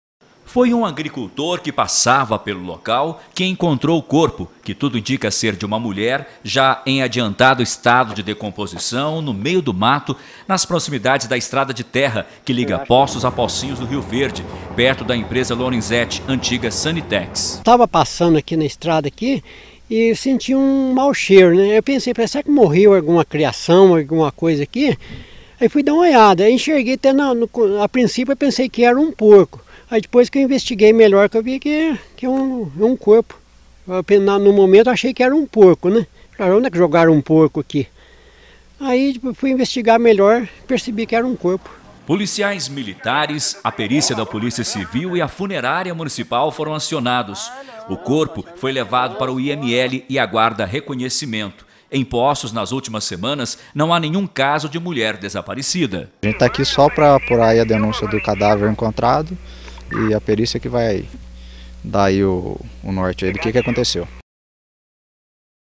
MULHER ENCONTRADA MORTA
Está no IML de Poços a espera de reconhecimento, o corpo de um mulher, encontrado em um matagal próxima a entrada antiga que liga Poços a Pocinhos do Rio Verde.